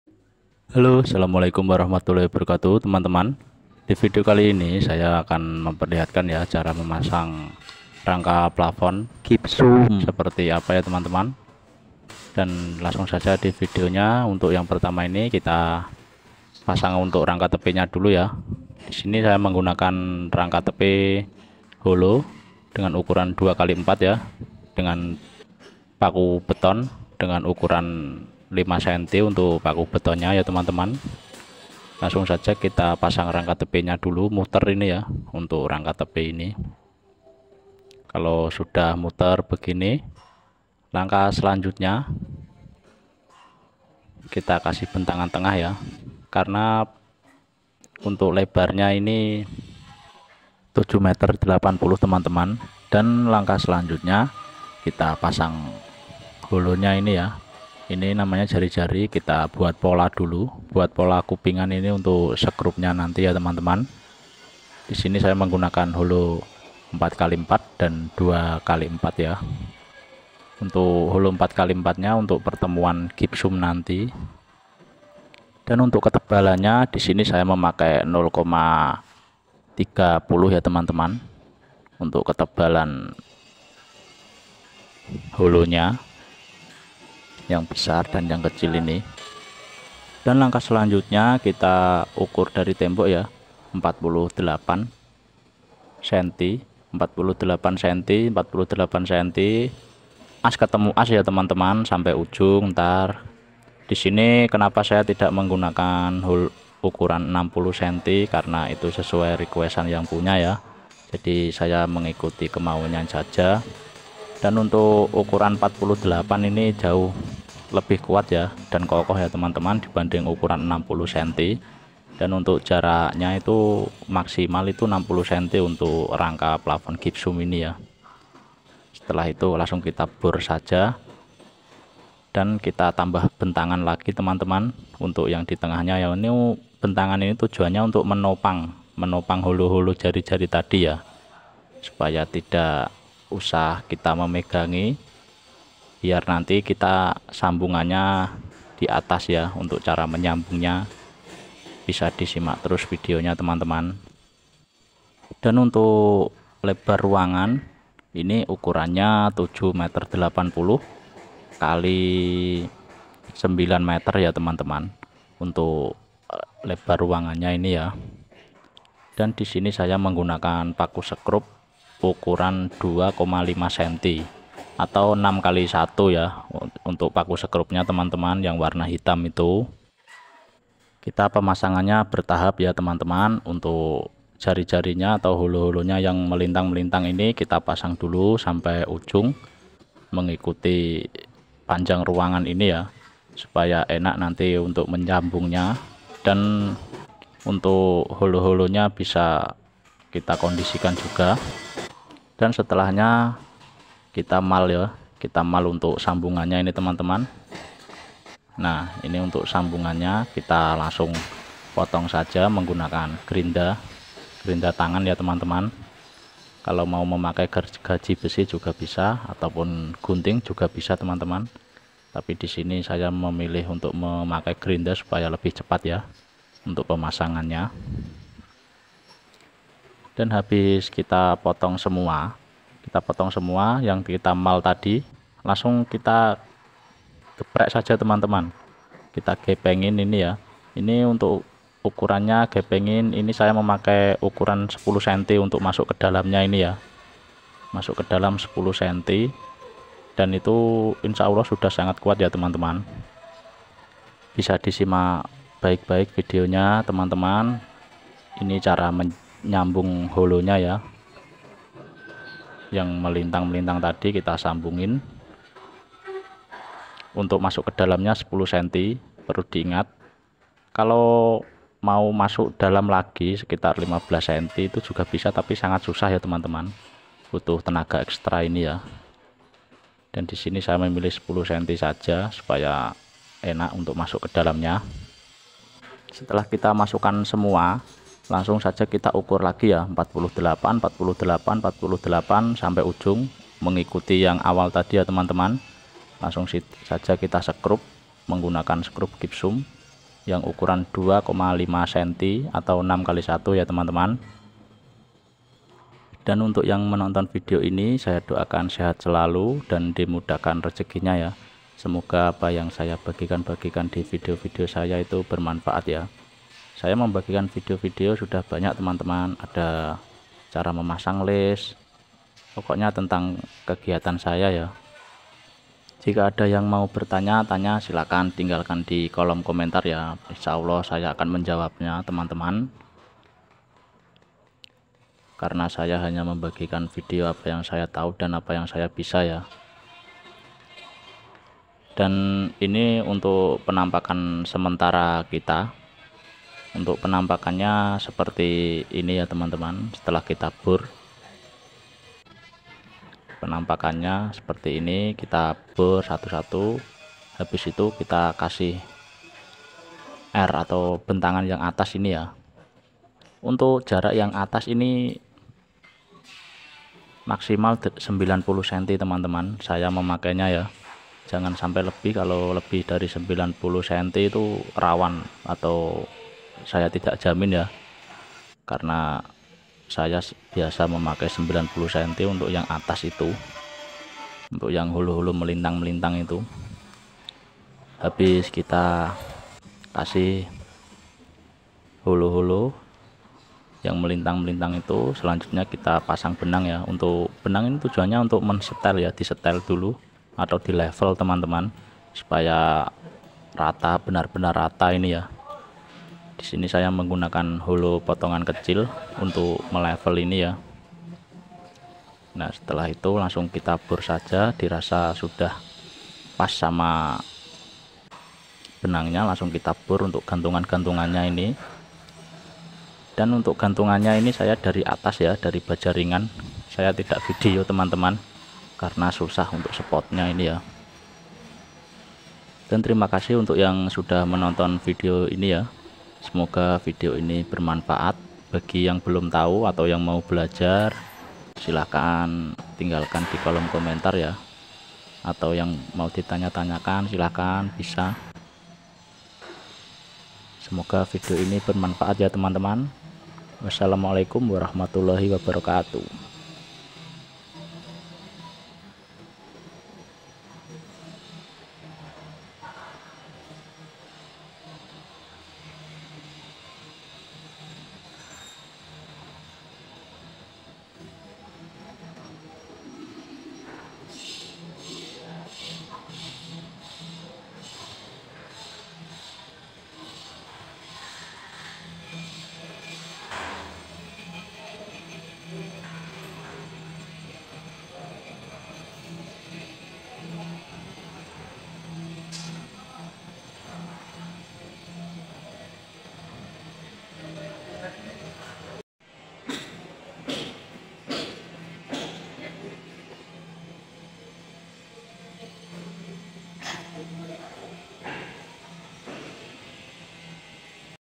jarang yang pakai cara ini // cara memasang rangka plafon gypsum
assalamualaikum teman teman dividio kali ini saya akan memberikan cara memasang rangka plafon gypsum untuk pemula dijamin langsung bisa ,,